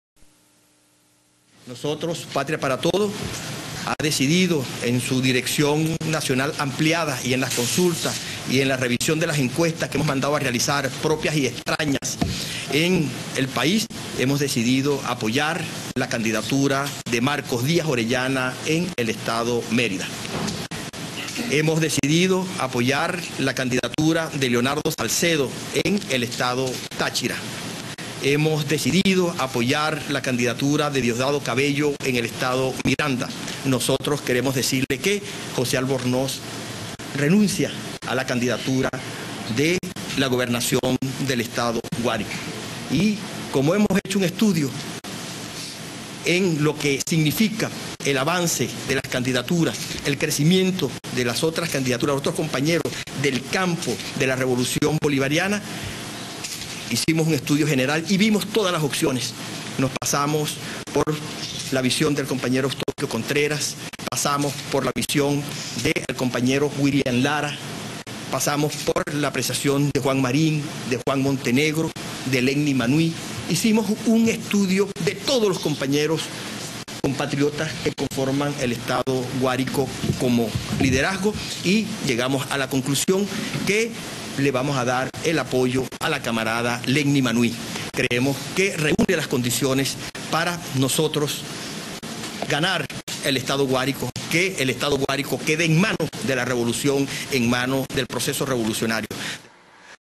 El PPT se enfrenta a Chávez
El Secretario General del PPT José Albornoz anunció que retiraba su aspiración como candidato a la gobernación del Guárico y que luego de hacer un estudio de las diversas opciones para esa región dijo que respaldarán a Lenny Manuitt, hija del actual gobernador y no a William Lara candidato del PSUV.